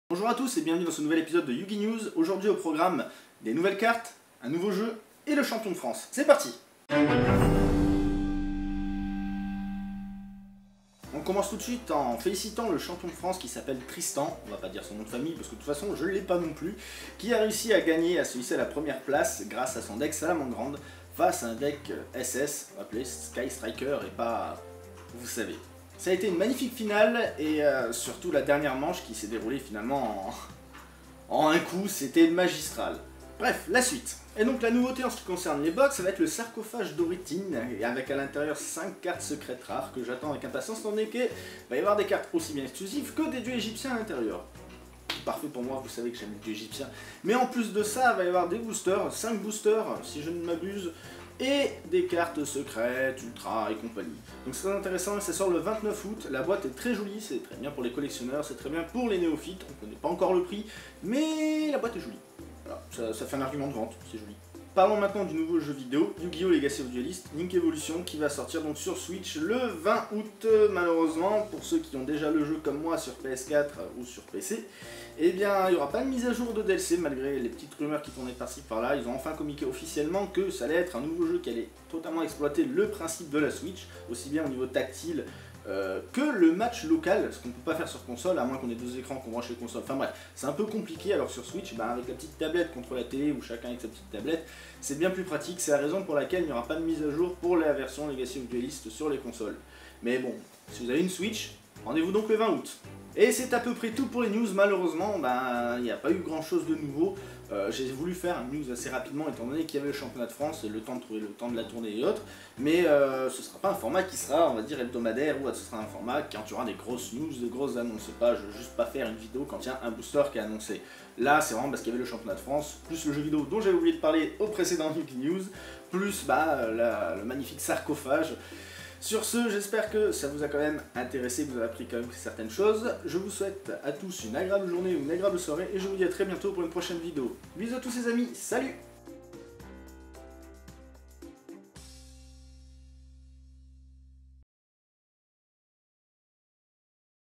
YUGI'NEWS #02 : UNE BOX ENORME ET LE CHAMPION DE FRANCE
Aujourd'hui on parle du champion de france, de la nouvelle box et du jeux vidéo !